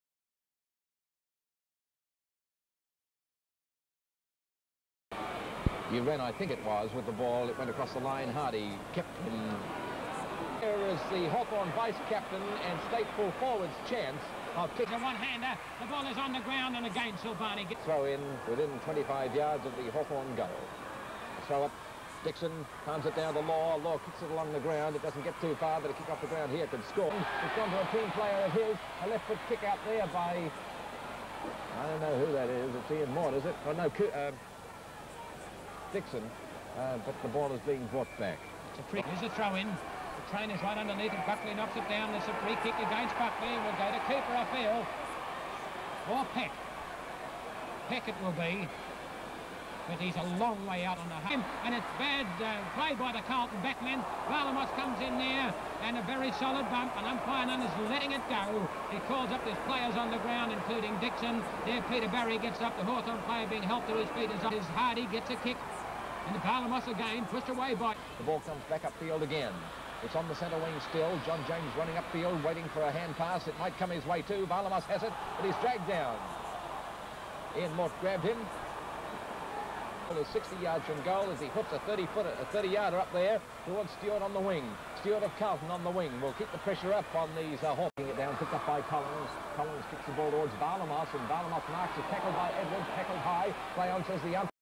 Vasil Varlamos - Carlton Football Club Past Player
This is the only video footage we have of 44 game player for the Carlton Football Club, number 32, Vasil Varlamos.

Vasil Varlamos was a rugged half-back flanker of Greek parentage who was a popular clubman during his five seasons at Princes Park from 1960 to '64. Born at Broken Hill, NSW, he was recruited by the Blues as a promising 17 year-old from bayside Mentone, and made his senior debut on the day after his 18th birthday in 1960.